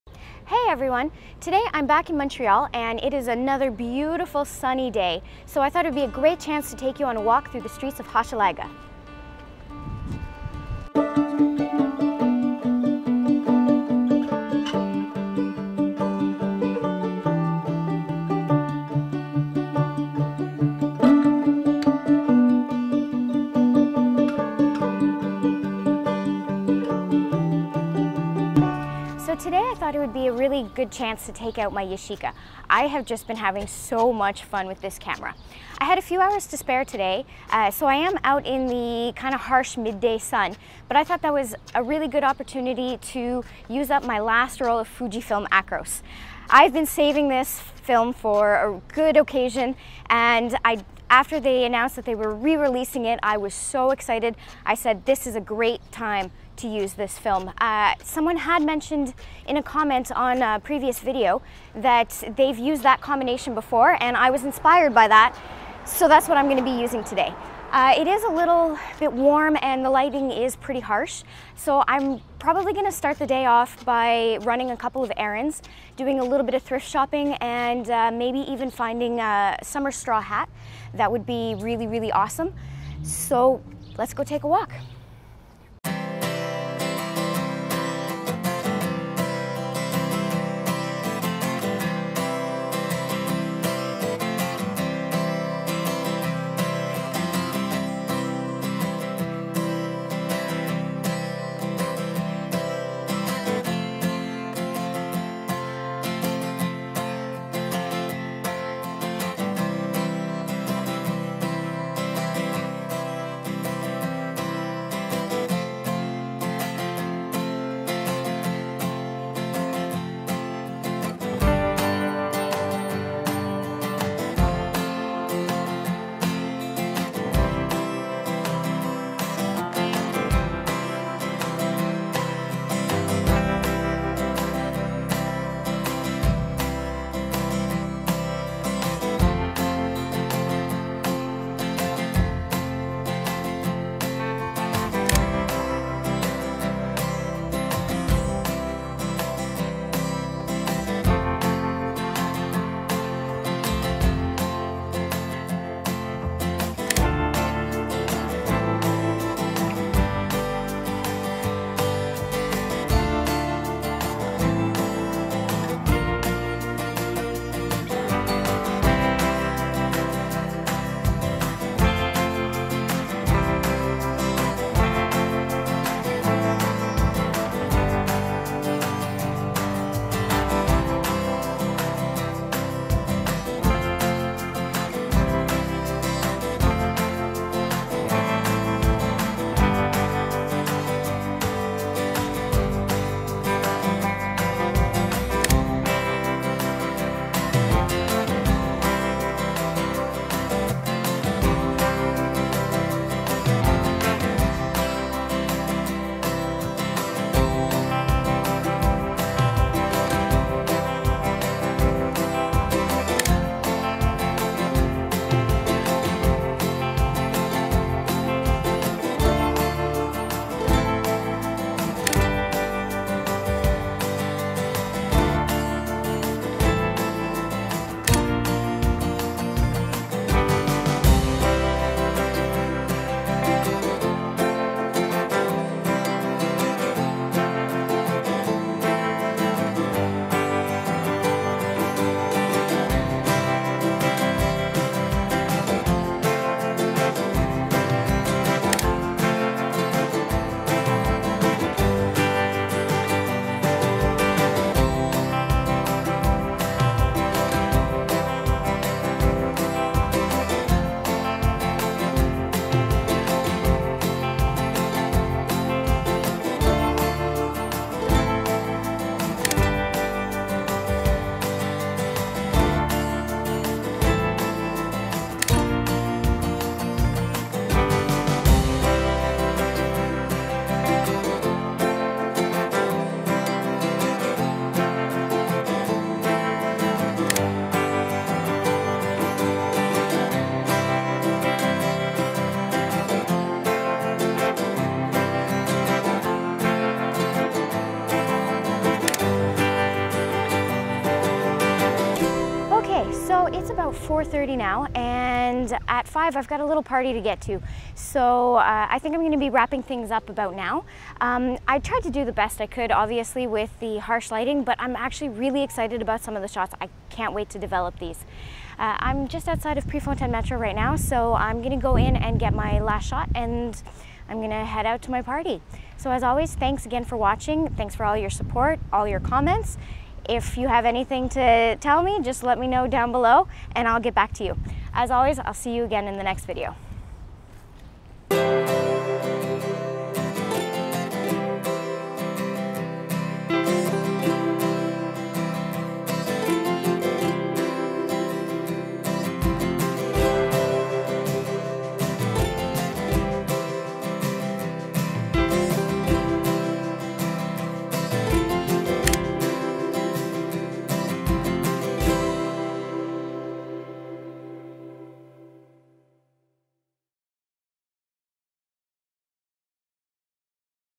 Hochelaga ||| Fujifilm Neopan Acros
A quick trip through Montreal's Hochelaga neighbourhood, hope you enjoy!!

As always, for a better look at some of the photos feel free to head on over to my Instagram page @jesshobbsphoto